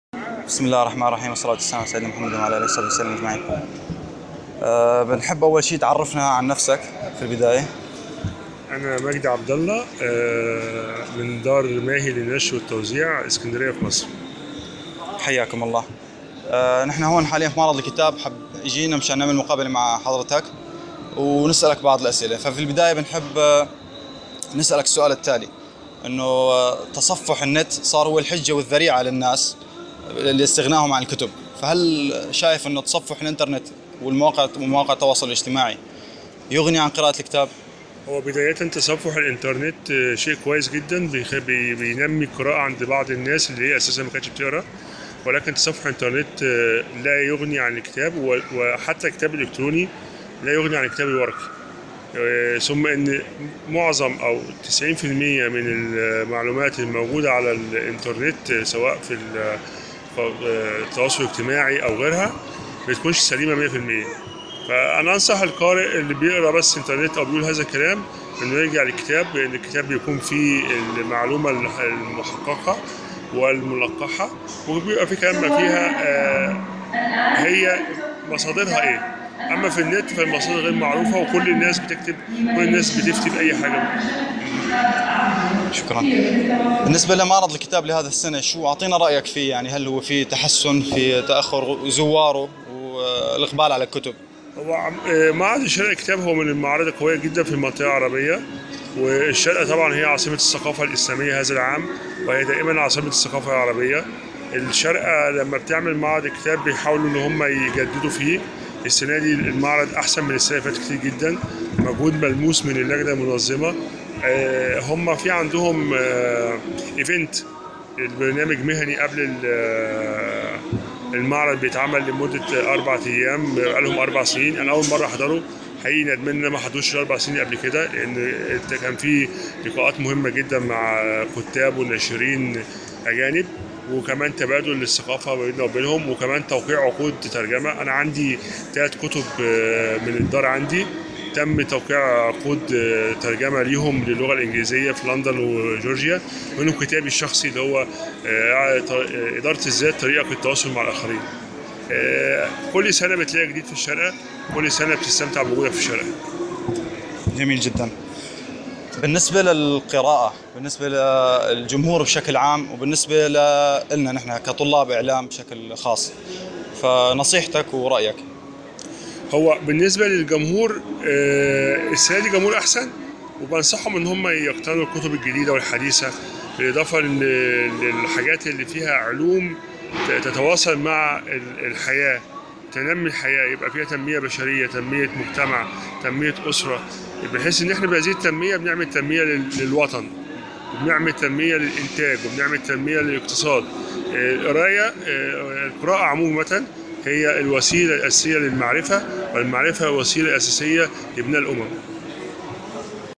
مقابلة مع المهندس مجدي محمد عبدالله
مقابلة مع المهندس مجدي محمد عبدالله تتنوال رأيه حول أهمية القراءة والكتب، إضافةً إلى رأيه حول معرض الكتاب لهذا العام (2014)، ويتطرّق المهندس مجدي إلى بعض مؤلّفاته وترجمتها.

المقابلة بتاريخ :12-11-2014 م \ يوم الأربعاء

#‏معرض_الشارقة_الدولي_للكتاب‬

تقديم ومونتاج : براء زرزور
المصوّرون : سعد ماهر عريقات _ محمد برنية _ محمد يوسف

‫#‏معرض_الشارقة_الدولي_للكتاب‬